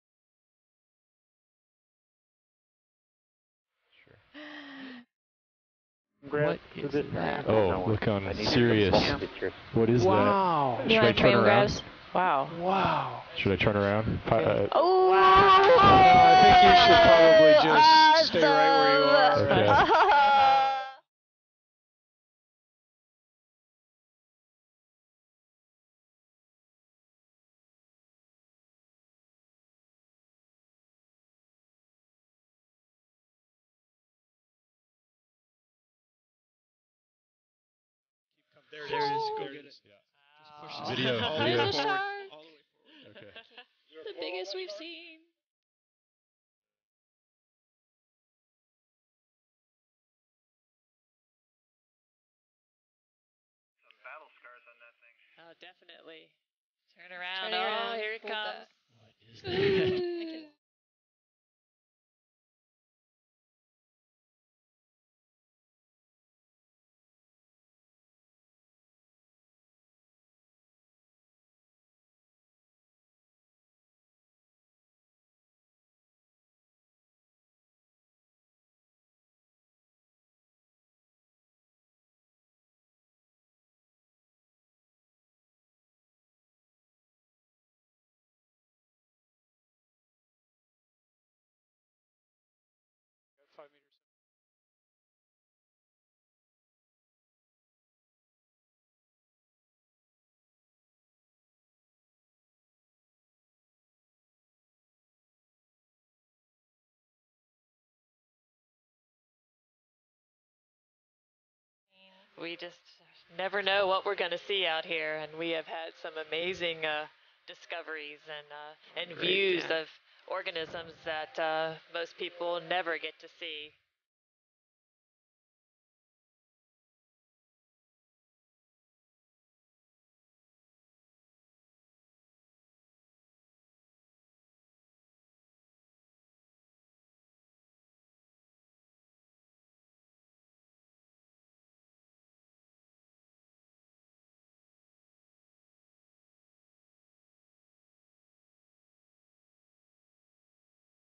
Greenland Shark: Northeast U.S. Canyons 2013
Did anyone see how the Okeanos Explorer 2013 field season ended in August?

In case you missed it, need a refresher, or want to relive the moment: Here's the Greenland shark that we encountered in the last few minutes of our last dive of the year off the Northeast U.S. coast.

To get a sense of the size of the shark, consider that our remotely operated vehicle has an overall length of 10.5 feet and stands an impressive 8.5 feet tall. This was a BIG shark and a great way to end the expedition...

Sit back, enjoy, and get ready -- we'll be back in April 2014 with more live video!

Video courtesy of the NOAA Office of Ocean Exploration and Research, Northeast U.S. Canyons 2013.